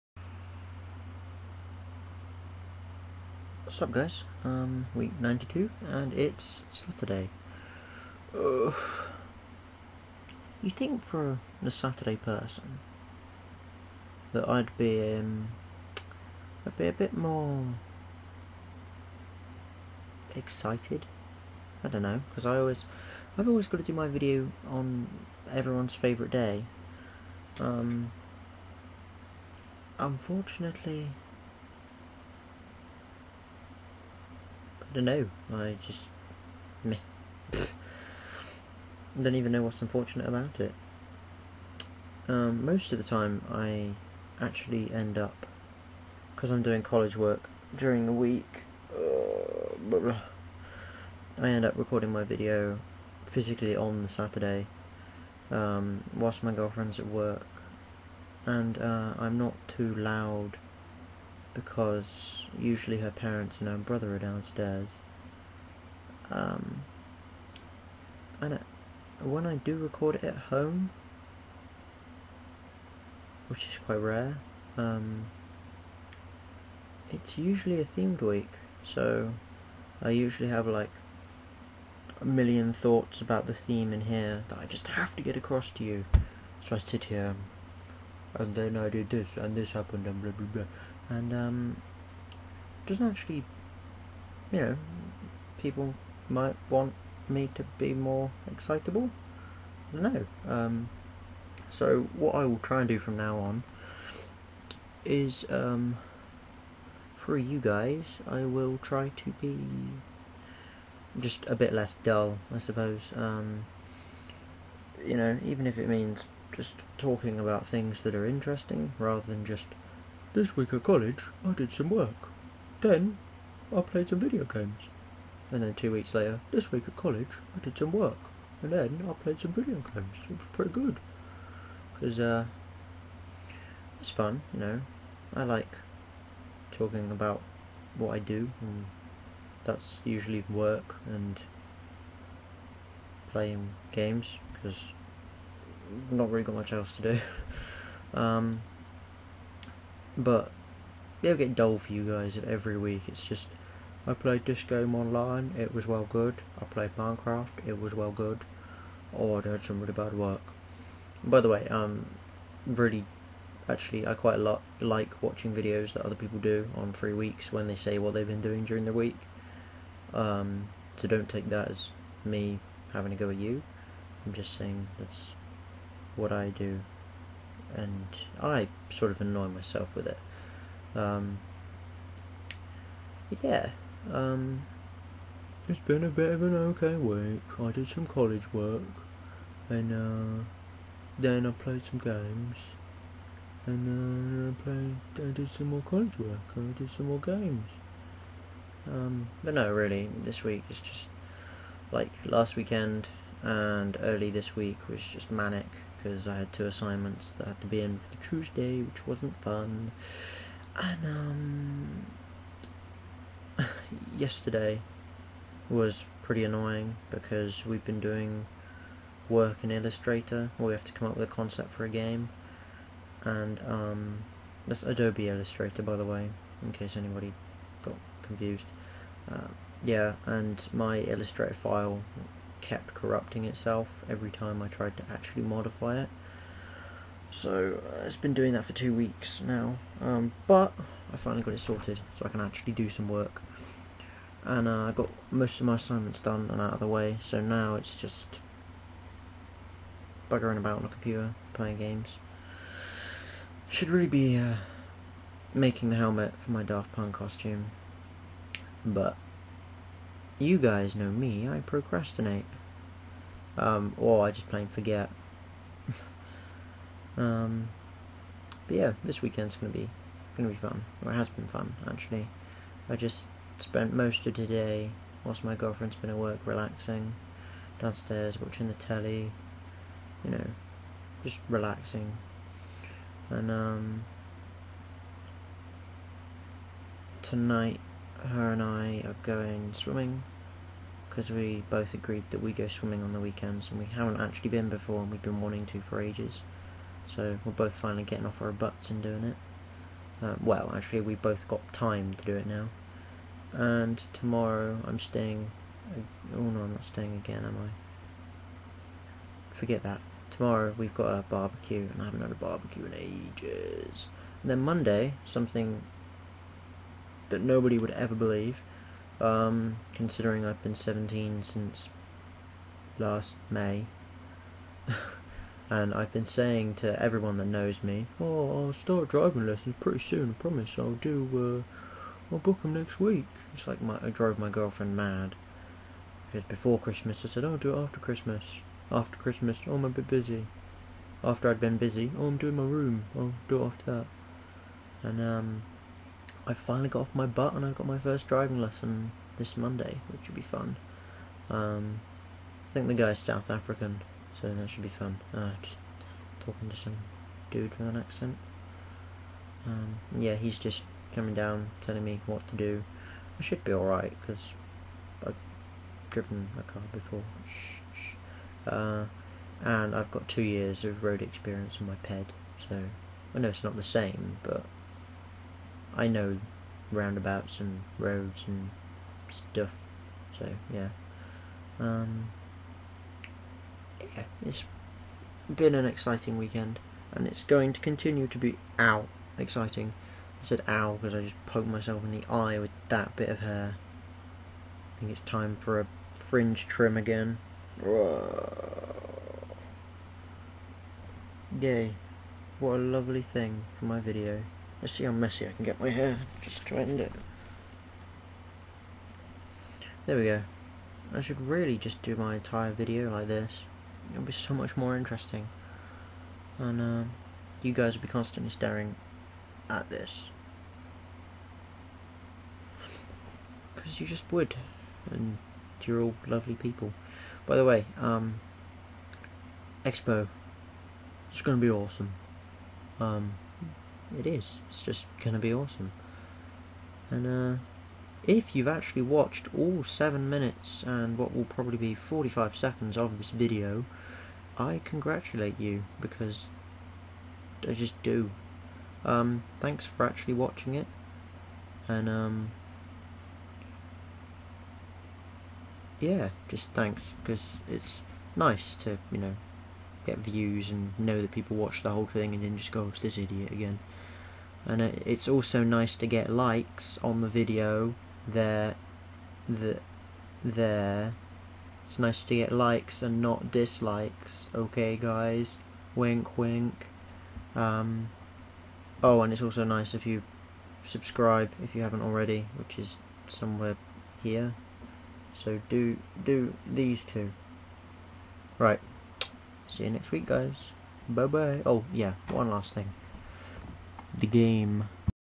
Driving lessons and crazy hair - J-H - Week 92
It's week 92. Free week. Just me chatting about how i  can improve my videos and what i've been up to this weekend :D